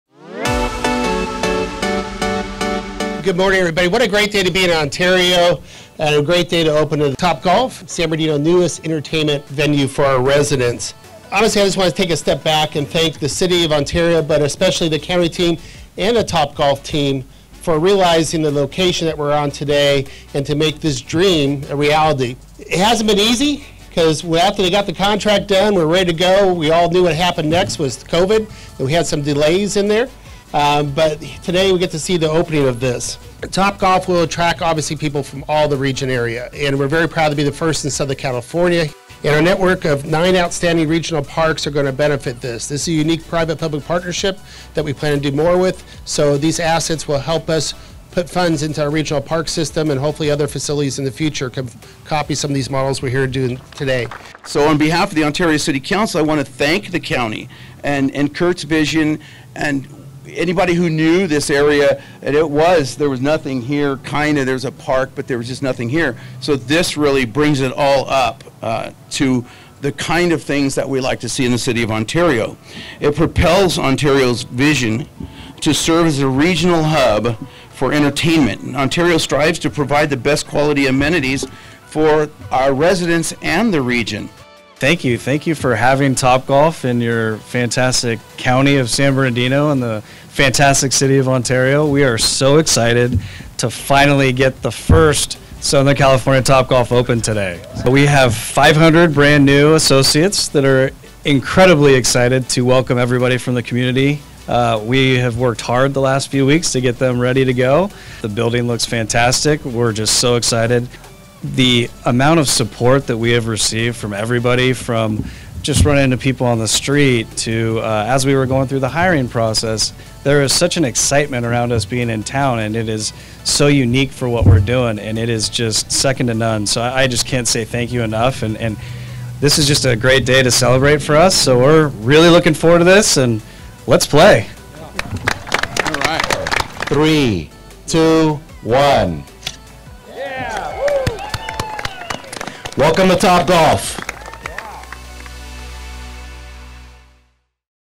San Bernardino County joins Topgolf to open Ontario location
There’s now one more reason why San Bernardino County is Southern California’s premiere recreation and business destination.
International entertainment phenomenon Topgolf opened its first Southern California location in Ontario Friday in a deal with San Bernardino County that will create approximately 400 private-sector jobs, $625,000 in annual revenue to County Regional Parks, and an additional recreational opportunity for families and businesses throughout the county and the region.
“Topgolf will be an outstanding entertainment asset that will attract people from all over Southern California to San Bernardino County,” Board of Supervisors Chairman Curt Hagman said as Topgolf Ontario opened its doors to the public.
Hagman, whose Fourth District includes the Topgolf site adjacent to Cucamonga-Guasti Regional Park in Ontario, developed the idea to use 13.7 acres of County-owned land at the southeast corner of Fourth Street and Archibald Avenue as a golf attraction more than five years ago. That led the County’s Real Estate Services Department to make contact with Topgolf.
“We are honored to be joining the San Bernardino County community as we open the doors to our newest venue to entertain the Inland Empire,” said Topgolf Chief Executive Officer Artie Starrs. “We are very excited to welcome the community to a fun, energetic experience one can only get at Topgolf.”
What started as a simple idea to enhance the game of golf has evolved into a best-in-class hospitality entertainment venue centered on a game that is accessible and appealing to all, regardless of skill or ability. The Topgolf technology-driven experience is centered on guests hitting patented, microchipped golf balls into targets on a field.
The open-air, three-level venue features 102 climate-controlled outdoor hitting bays, chef-inspired signature menu items, top-shelf drinks, music, and year-round programming for all ages. The venue is equipped with an outdoor patio, rooftop terrace and private event space.
Topgolf has 75 venues in 31 U.S. states and international locations in Dubai, the United Kingdom, Australia and Germany.
“The Topgolf concept appeals to all ages, and everyone will benefit from the revenue that will be directed toward the betterment of the county’s Regional Parks system,” Hagman said.
Over the course of the initial 20-year ground lease between the County and Topgolf, the County will receive more than $625,000 in annual revenue, benefiting the San Bernardino County Regional Parks system, which includes nine regional parks throughout the county and several other recreational attractions.
Those interested in joining the team at Topgolf can visit Topgolf’s career website.